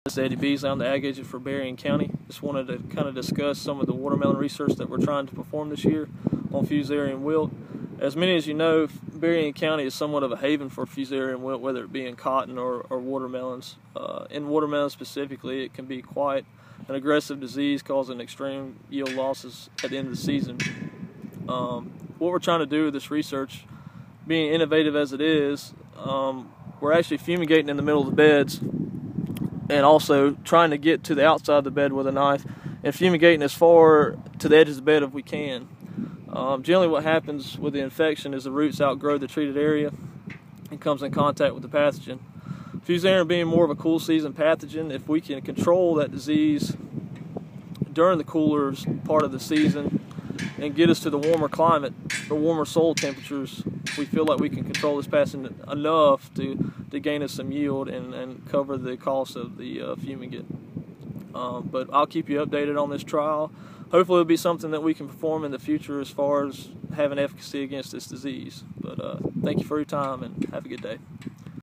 Fusarium Wilt Research in Watermelon
Fusarium Wilt is a serious soil borne disease of watermelon which can cause extreme yield losses where it is prevalent. As of right now, there is no "silver bullet" for management of this disease, so new innovative options are being put to the test. Fumigation techniques using methods to apply fumigants near the shoulder of the bed may extend the treated area and inhibit roots coming in contact with the pathogen.